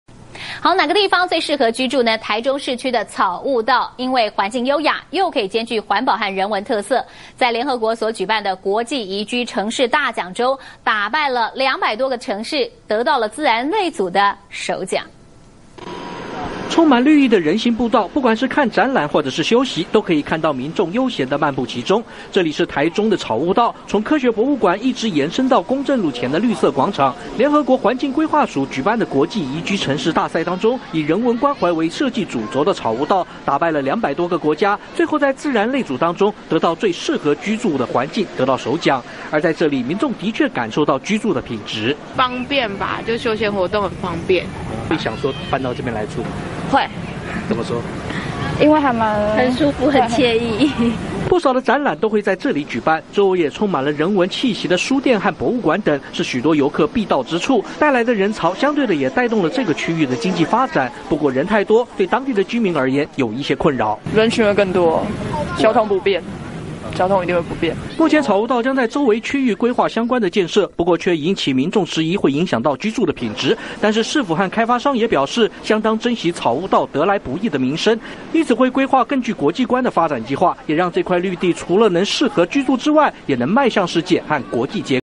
中視新聞》「草悟道」自然藝術兼備 奪全球自然類金牌
內容轉載自《中時電子報》：
中視新聞》台中市區的草悟道因為環境優雅，又能兼具環保和人文特色，在聯合國舉辦的國際宜居城市大獎中，打敗了其他兩百多個城市，得到了自然類組的首獎，而這一條充滿綠色的公園環道，也漸漸成為許多遊客到台中一定要去的景點。